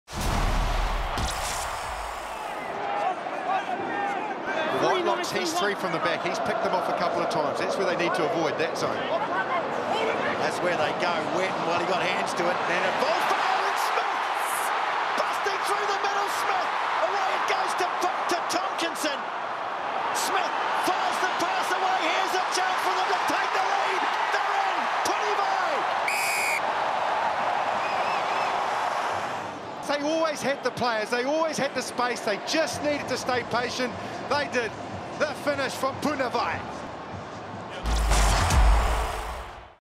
TRY OF THE WEEK | Super Rugby Aotearoa Rd 4
It was a supreme team try from the Highlanders all started by Aaron Smith off the back of a lineout, finished by Ngane Punivai.